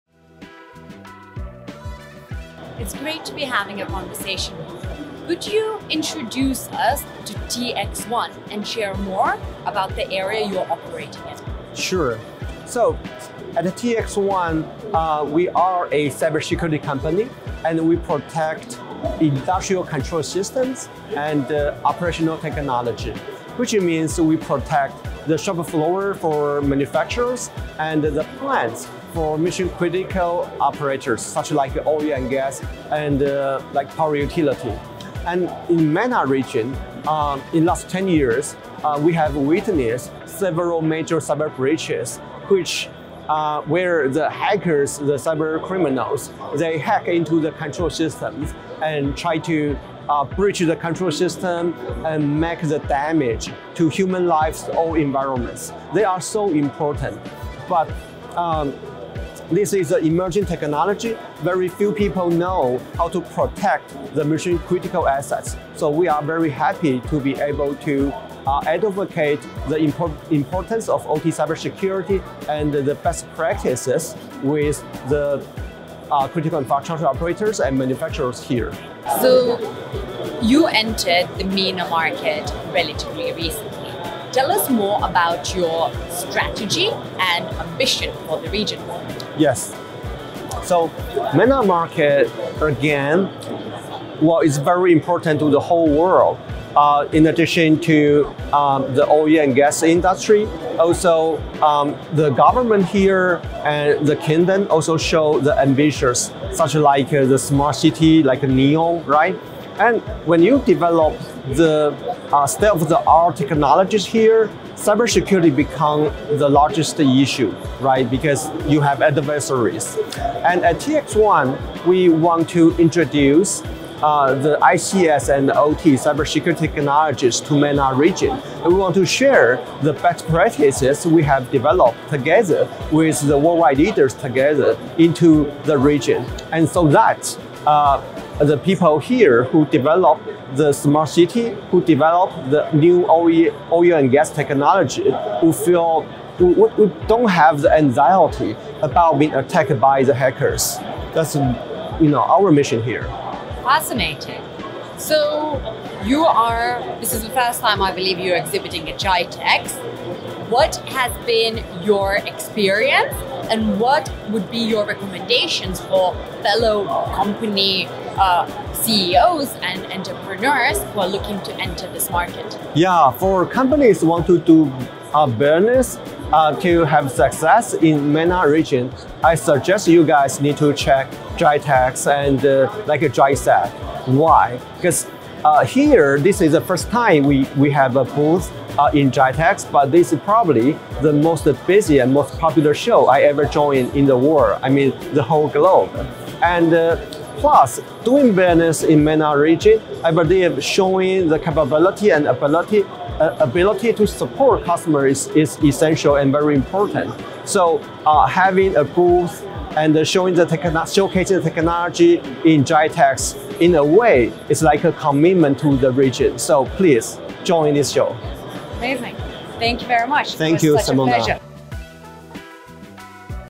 EntMETalks with TXOne co-founder and CEO Dr. Terence Liu
Smart cities rely on operational technology (OT) systems to control critical infrastructure, such as transportation systems, power grids, and water systems. It is those OT systems that are often vulnerable to cyberattacks which can lead to devastating consequences.
The importance of OT cybersecurity was therefore one of the topics for our interview with Dr. Terence Liu, co-founder and CEO of TXOne, at the 2023 GITEX GLOBAL.